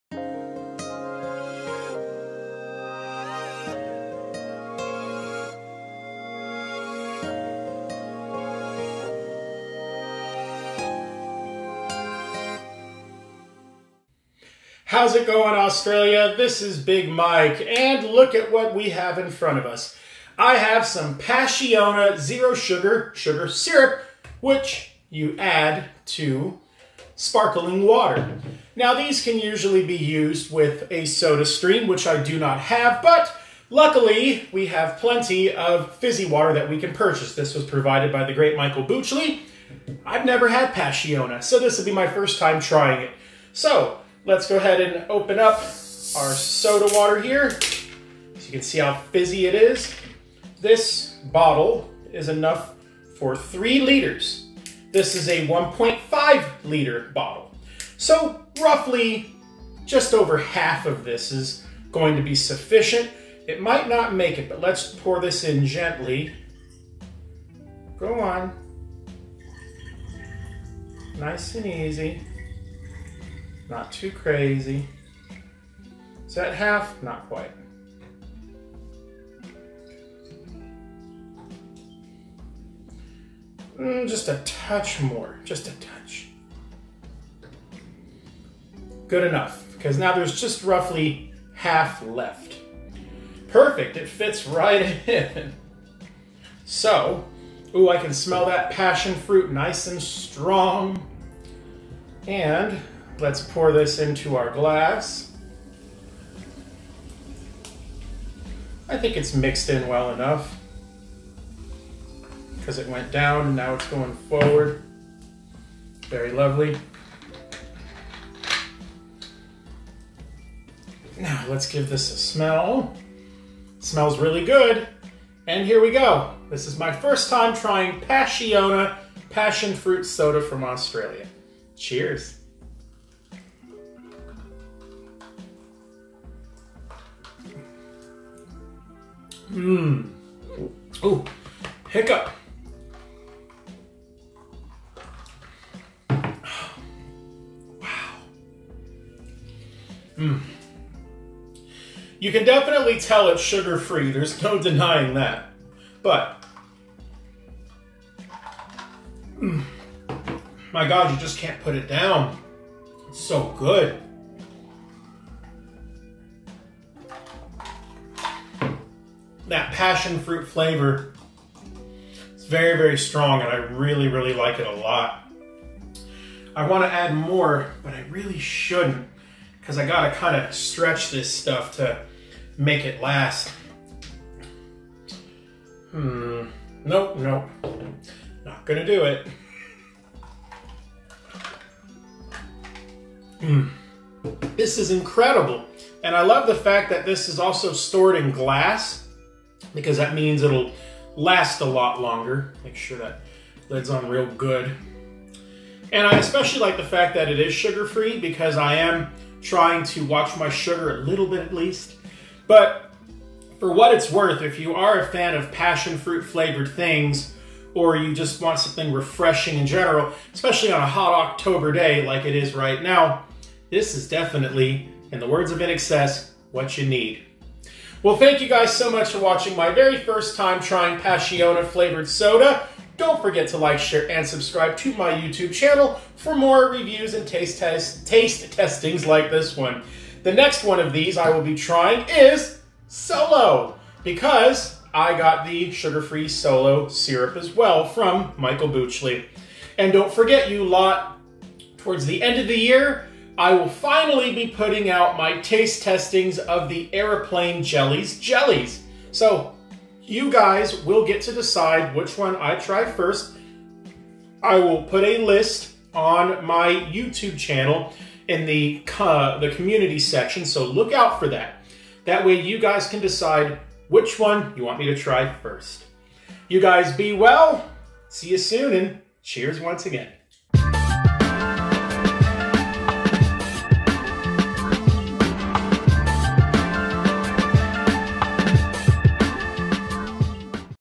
Passiona Soda Taste Test
My first time trying the Australian classic soda Passiona!